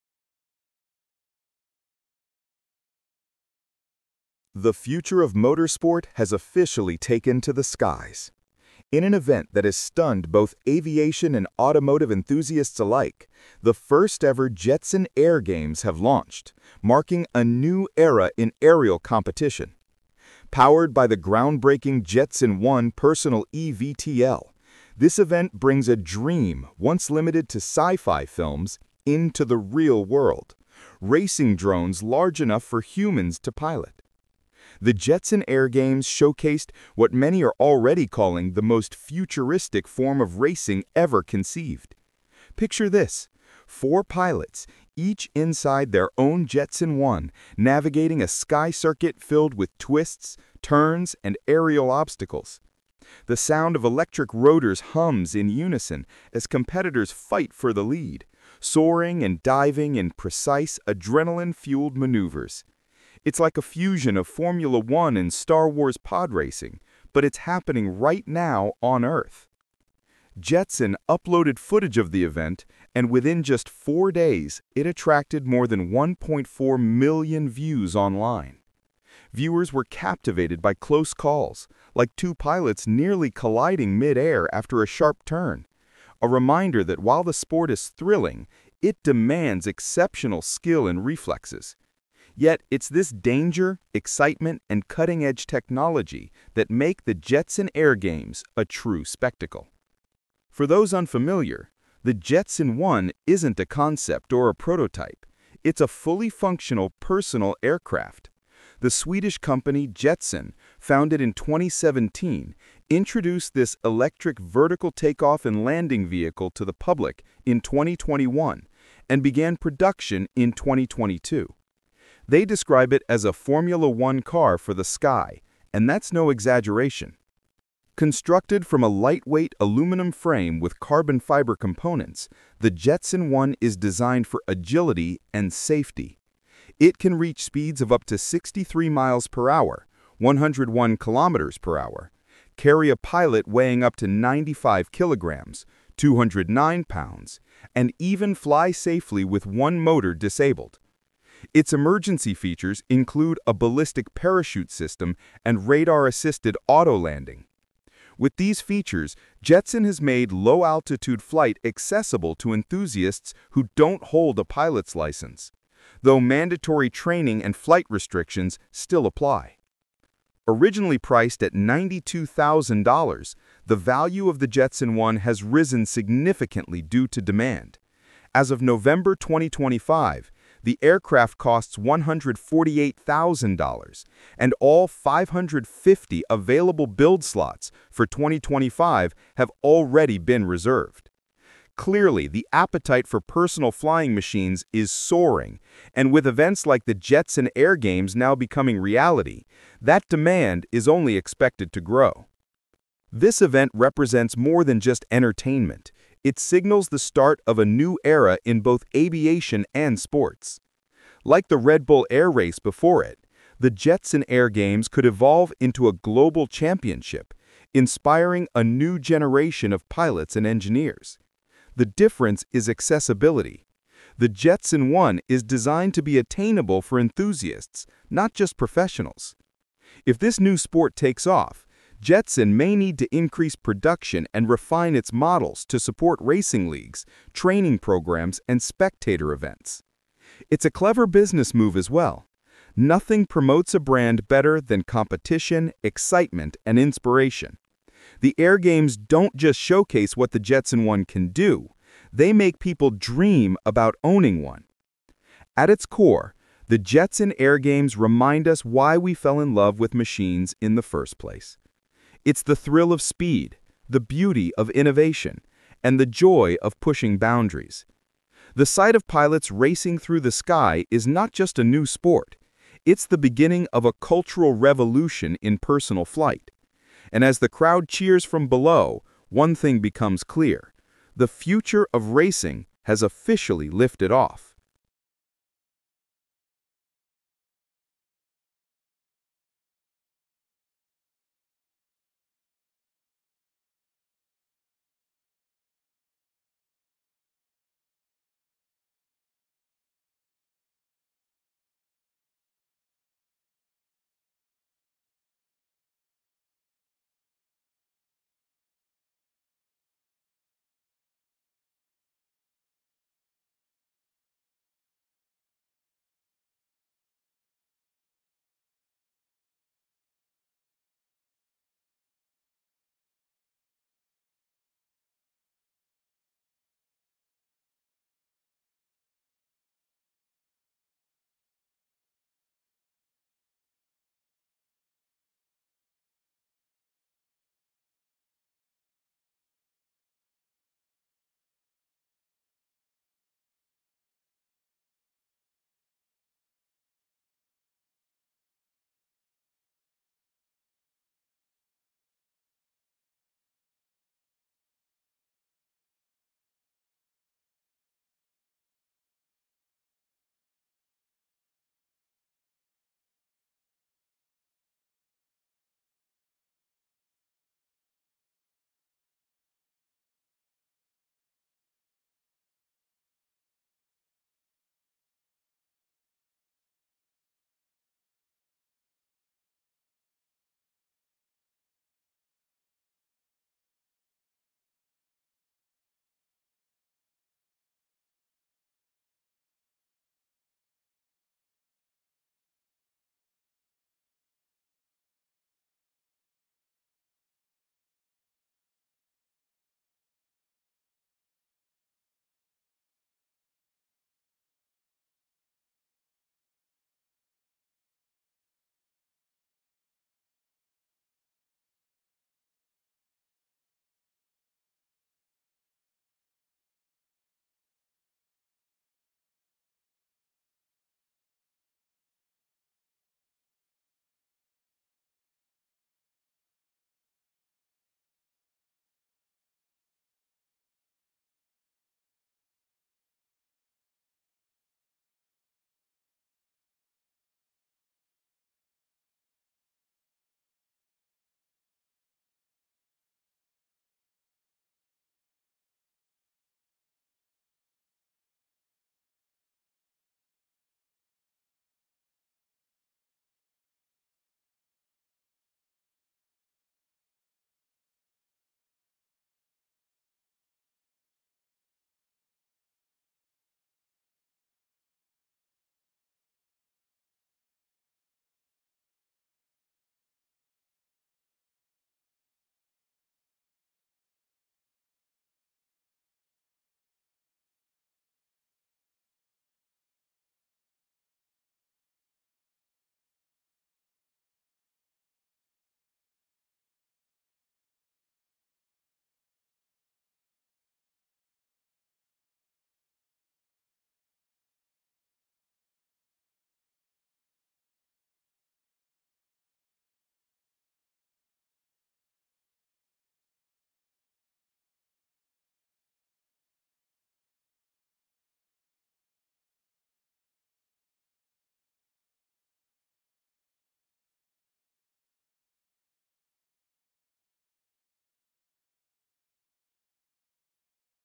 Jetson Air Games Take Flight: The World’s First eVTOL Racing League Begins With the Jetson One
The future of motorsport has officially taken to the skies. In an event that has stunned both aviation and automotive enthusiasts alike, the first-ever Jetson Air Games have launched, marking a new era in aerial competition. Powered by the groundbreaking Jetson One personal eVTOL, this event brings a dream once limited to sci-fi films into the real world — racing drones large enough for humans to pilot.

The Jetson Air Games showcased what many are already calling the most futuristic form of racing ever conceived. Picture this: four pilots, each inside their own Jetson One, navigating a sky circuit filled with twists, turns, and aerial obstacles. The sound of electric rotors hums in unison as competitors fight for the lead, soaring and diving in precise, adrenaline-fueled maneuvers. It’s like a fusion of Formula One and Star Wars Podracing — but it’s happening right now on Earth.

Jetson uploaded footage of the event, and within just four days, it attracted more than 1.4 million views online. Viewers were captivated by close calls, like two pilots nearly colliding mid-air after a sharp turn — a reminder that while the sport is thrilling, it demands exceptional skill and reflexes. Yet, it’s this danger, excitement, and cutting-edge technology that make the Jetson Air Games a true spectacle.

For those unfamiliar, the Jetson One isn’t a concept or a prototype — it’s a fully functional personal aircraft. The Swedish company Jetson, founded in 2017, introduced this electric vertical take-off and landing vehicle to the public in 2021 and began production in 2022. They describe it as “a Formula One car for the sky,” and that’s no exaggeration.

Constructed from a lightweight aluminum frame with carbon fiber components, the Jetson One is designed for agility and safety. It can reach speeds of up to 63 mph (101 km/h), carry a pilot weighing up to 95 kilograms (209 pounds), and even fly safely with one motor disabled. Its emergency features include a ballistic parachute system and radar-assisted auto-landing. With these features, Jetson has made low-altitude flight accessible to enthusiasts who don’t hold a pilot’s license — though mandatory training and flight restrictions still apply.

Originally priced at $92,000, the Jetson One’s value has risen significantly due to demand. As of November 2025, the aircraft costs $148,000, and all 550 available build slots for 2025 have already been reserved. Clearly, the appetite for personal flying machines is soaring, and with events like the Jetson Air Games now becoming reality, that demand is only expected to grow.

This event represents more than just entertainment; it signals the start of a new era in both aviation and sports. Like the Red Bull Air Race before it, the Jetson Air Games could evolve into a global championship, inspiring a new generation of pilots and engineers. The difference is accessibility — the Jetson One is designed to be attainable for enthusiasts, not just professionals.

If this new sport takes off, Jetson may need to increase production and refine its models to support racing leagues, training programs, and spectator events. It’s a clever business move as well — nothing promotes a brand better than competition, excitement, and inspiration. The Air Games don’t just showcase what the Jetson One can do; they make people dream about owning one.

At its core, the Jetson Air Games remind us why we fell in love with machines in the first place. It’s the thrill of speed, the beauty of innovation, and the joy of pushing boundaries. The sight of pilots racing through the sky is not just a new sport — it’s the beginning of a cultural revolution in personal flight. And as the crowd cheers from below, one thing becomes clear: the future of racing has officially lifted off.